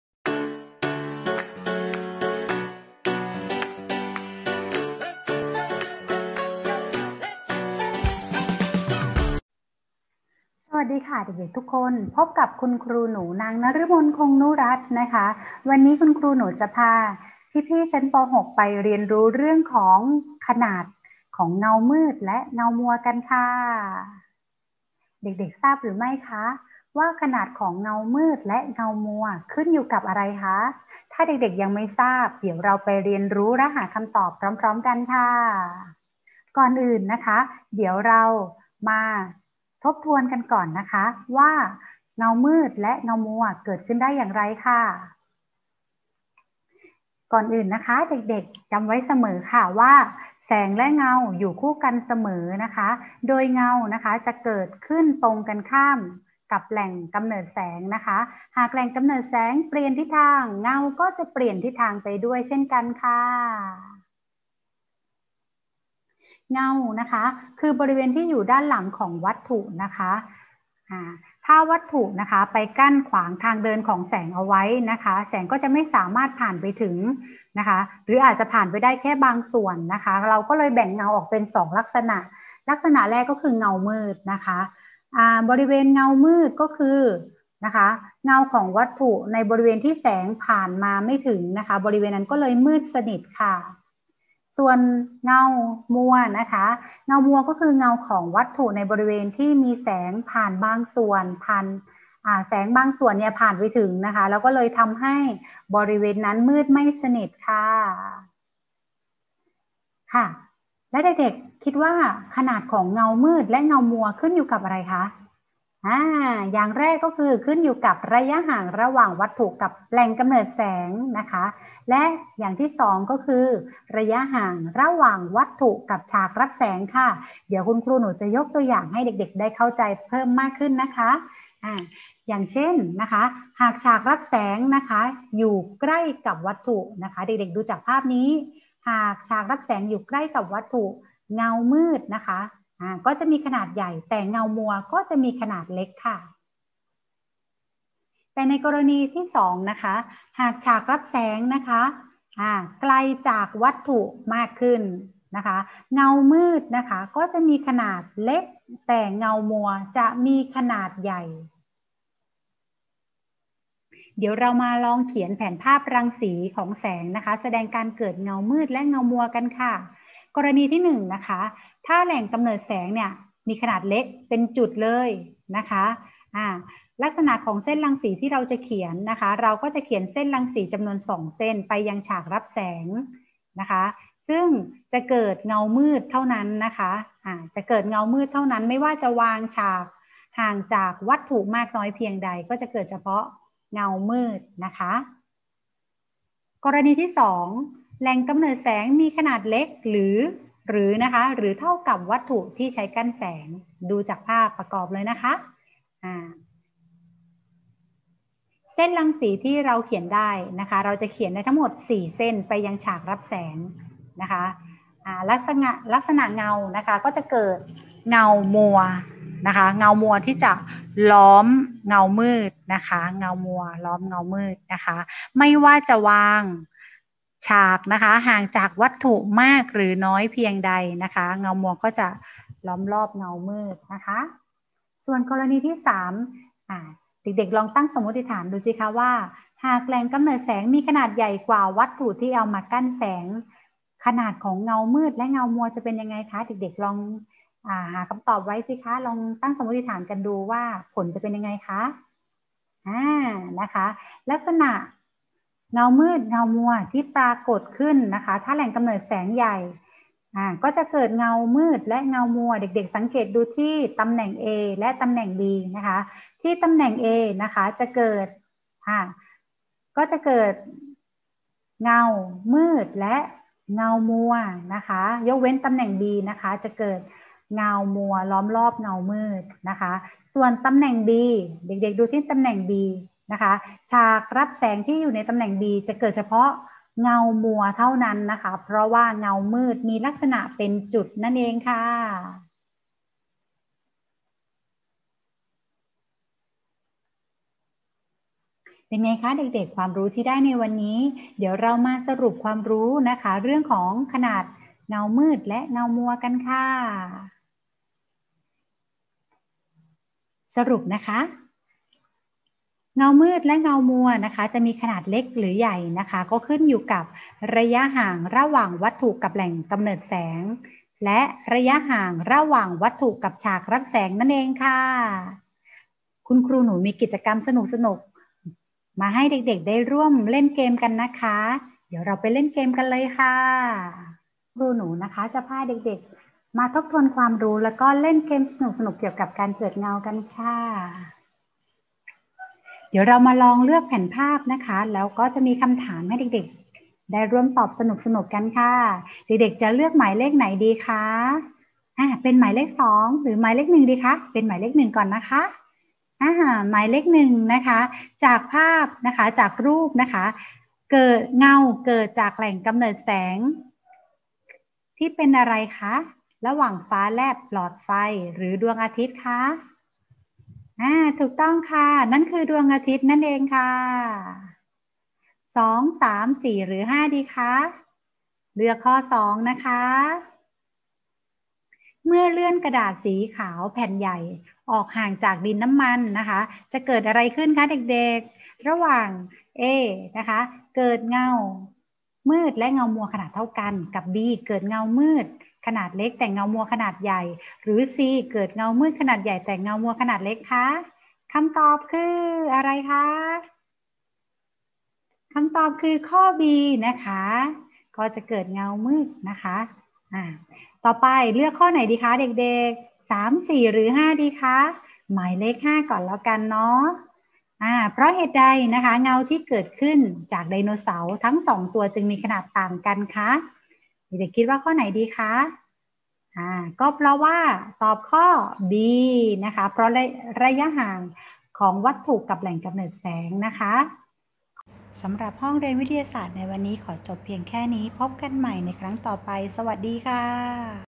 วิชาวิทยาศาสตร์ ป.6 เรื่องขนาดของเงามืดและเงามัว
เด็ก ๆ ทราบหรือไม่ว่าขนาดของเงามืดและเงามัวสามารถเปลี่ยนแปลงได้ อะไรเป็นปัจจัยที่ทำให้เงาเปลี่ยนขนาดได้ เราไปเรียนรู้พร้อม ๆ กันเลยค่ะ